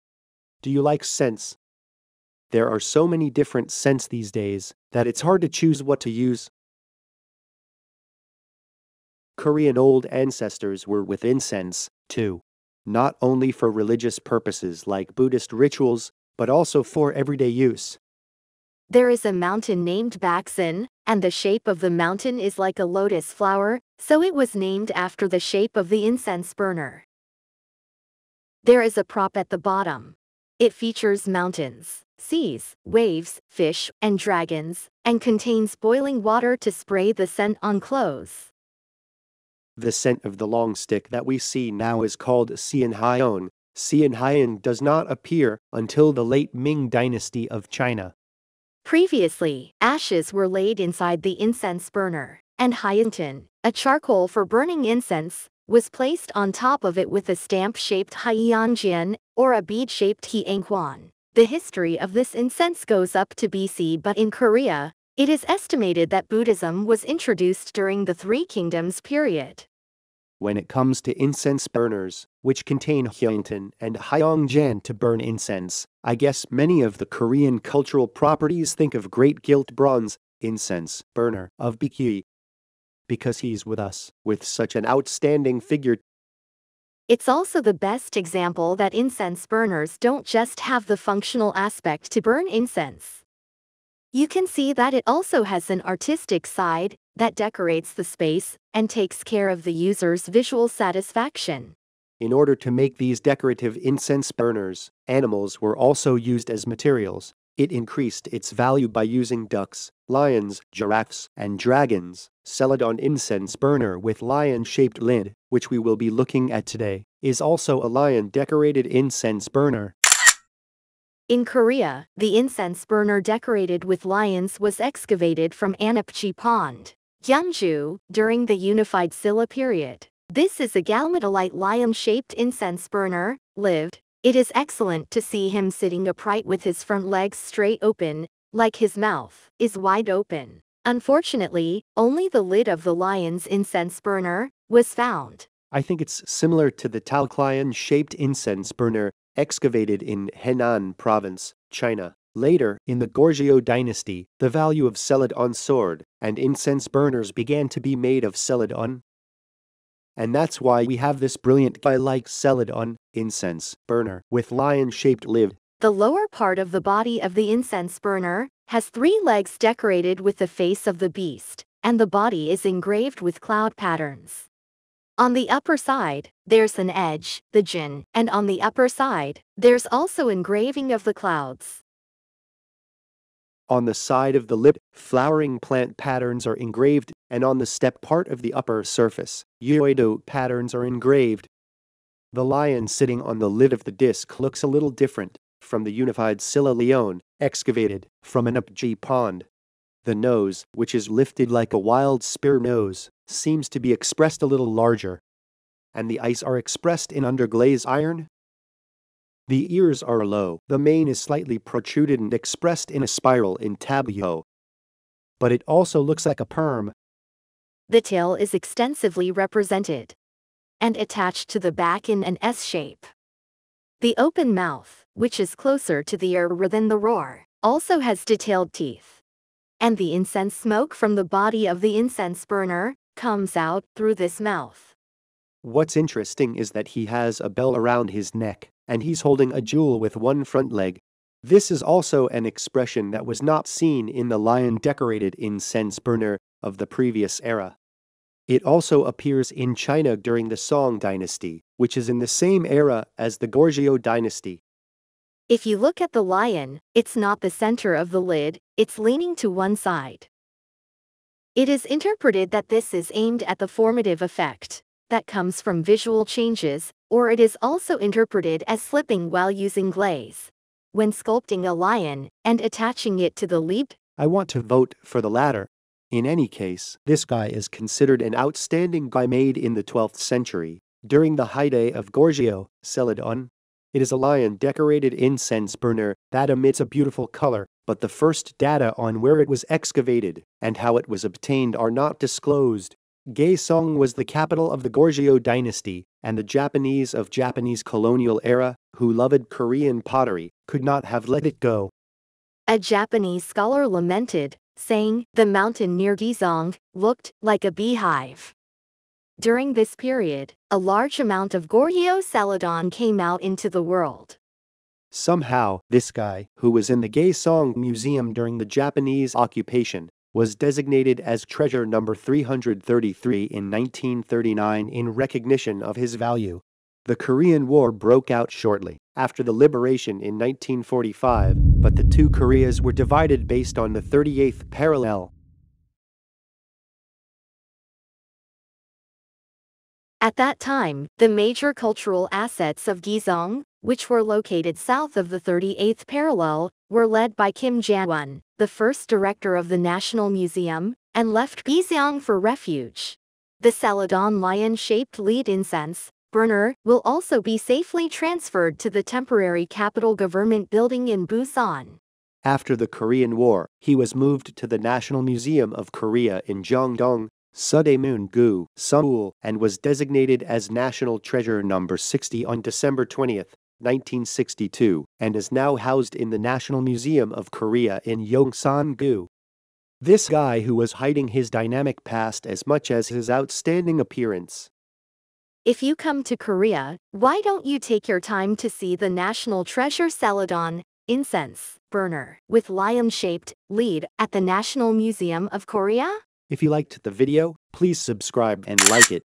The Story of a Lion Who Escaped the Korean War - Celadon Incense Burner with Lion-shaped Lid
It is believed to have been made during the heyday of Goryeo celadon in the 12th century
The national treasure Celadon Incense Burner with Lion-shaped Lid still has a brilliant color.
Are you curious about the story of how this guy in Gaeseong escaped the Korean War and became a national treasure of South Korea?

* 참고자료
- 국립중앙박물관, “고려시대 향로”, 2013
- 강경남, ‘청자사자장식향로에 관한 시론적 고찰’, “동원학술논문집”11, 2010
- 彭善国, ‘渤海上京城2 号宫殿址出土玉石 “杖首”应为“熏炉”考辨’, “北方文物”, 2017
- 국립중앙박물관 유투브 : [듣고보는 국립중앙박물관 소장품] 청자 사자 장식 향로